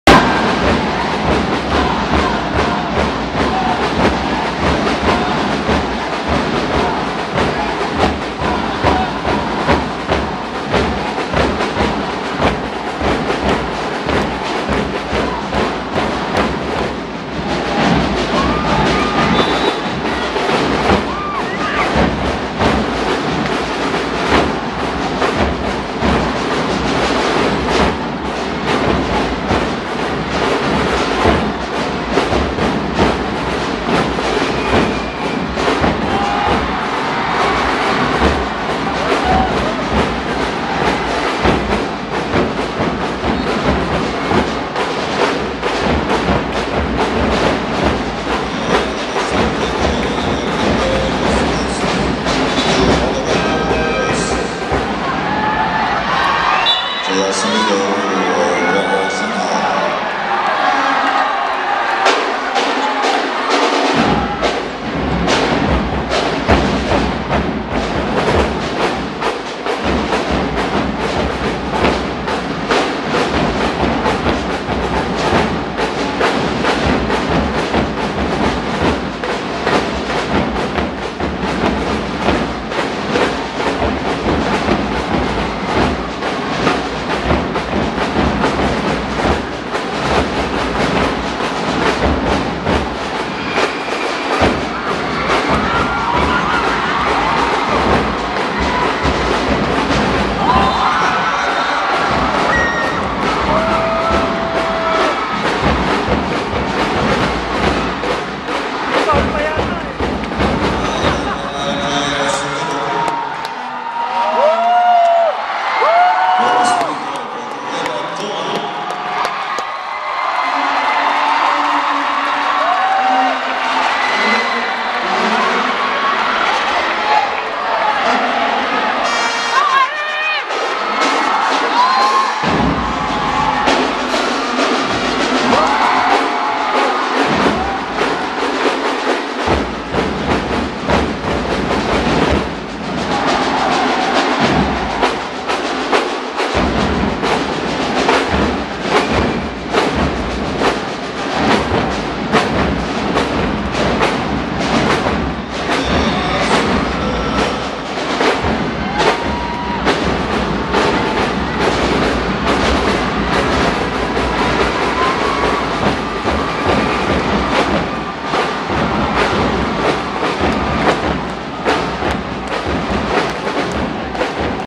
UAAP 2011 UST vs UE
September 28, 2011  UAAP 2011  the cheering, the drums, the rebounds, the heart, the win!  The UST Tigers growled 54-45!